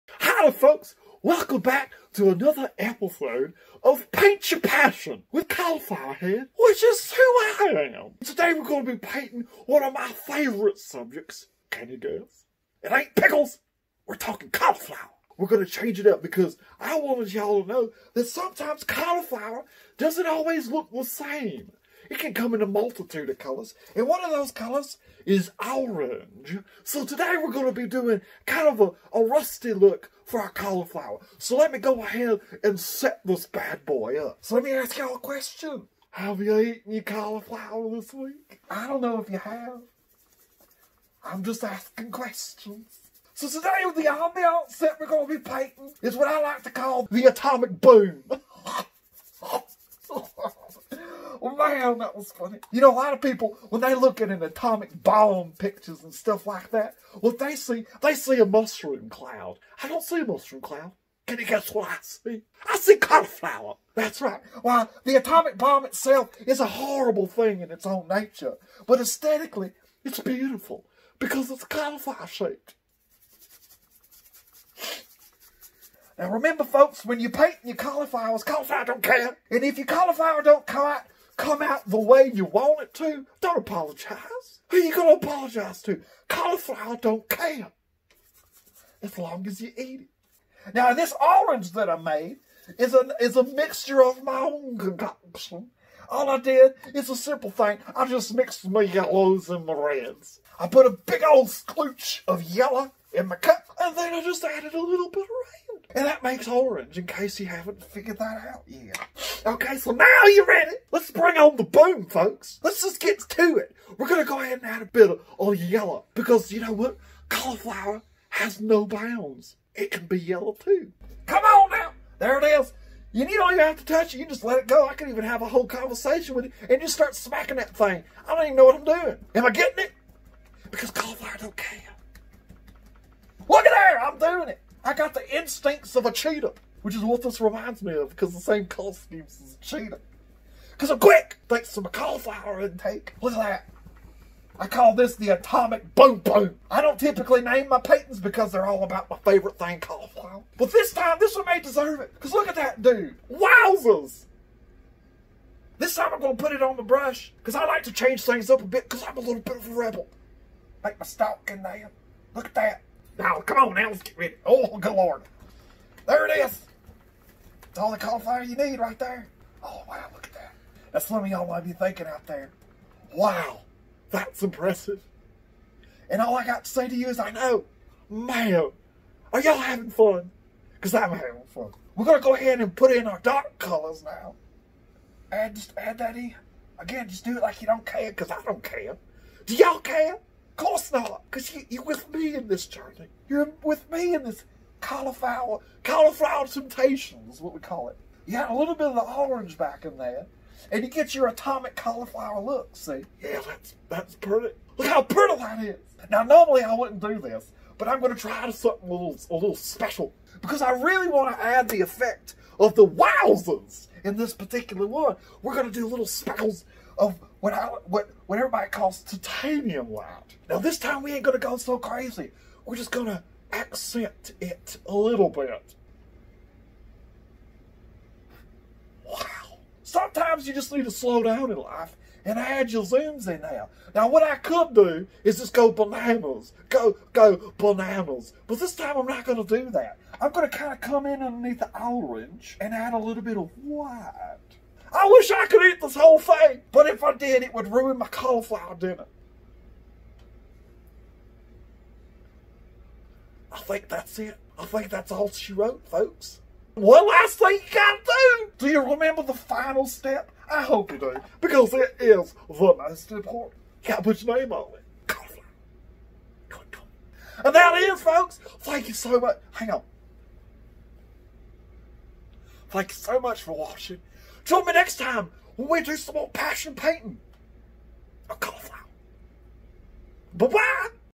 Cauliflower Head - Atomic Boom
Cauliflower changes things up in this video, but not by much.  When all you think about is cauliflower, you tend to see cauliflower in everything!  Enjoy.